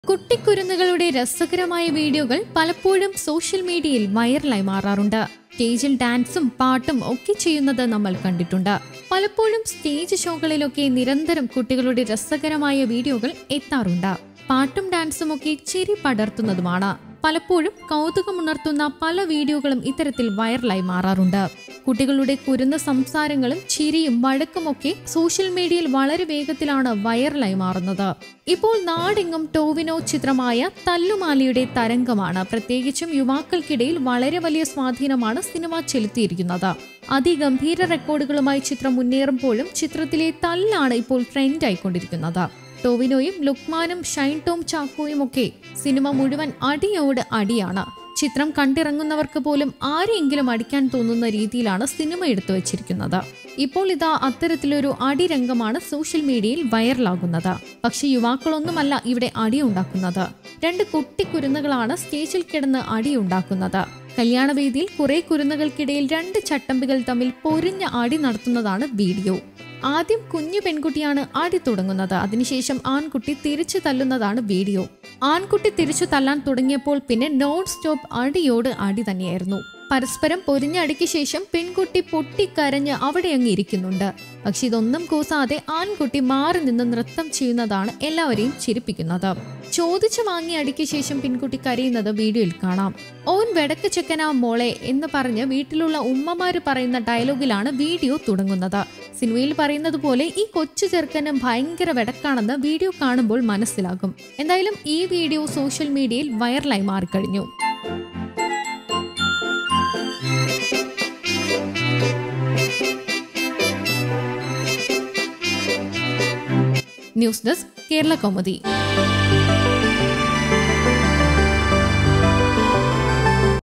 സ്റ്റേജിൽ കുരുന്നുകളുടെ കൂട്ടത്തല്ല് ചിരിച്ച് മരിക്കും | Kids fighting | Funny Video
കുട്ടിക്കരുന്നുകളുടെ രസകരമായ വീഡിയോകൾ പലപ്പോഴും സോഷ്യൽ മീഡിയയിൽ വൈറലാകാറുണ്ട്. സ്‌റ്റേജിൽ ഡാൻസും പാട്ടും ഒക്കെ ചെയ്യുന്നത് നമ്മൾ കണ്ടിട്ടുണ്ട്. പലപ്പോഴും സ്‌റ്റേജ് ഷോകളിൽ ഒക്കെ നിരന്തരം കുട്ടികളുടെ രസകരമായ വീഡിയോകൾ എത്താറുണ്ട്. പാട്ടും ഡാൻസും ഒക്കെ ചിരി പടർത്തുന്നതാണ്. നാടെങ്ങും ഇപ്പോൾ  ടൊവിനോ ടൊവിനോ ചിത്രമായ തല്ലുമാലയുടെ തരംഗമാണ്. പ്രത്യേകിച്ച് യുവാക്കൾക്കിടയിൽ വളരെ വലിയ സ്വാധീനമാണ് സിനിമ ചെലുത്തിയിരിക്കുന്നത്. അതിഗംഭീര റെക്കോർഡുകളുമായി ചിത്രം മുന്നേറുമ്പോഴും ചിത്രത്തിലെ തല്ലാണ് ഇപ്പോൾ ട്രെൻഡായി കൊണ്ടിരിക്കുന്നത്.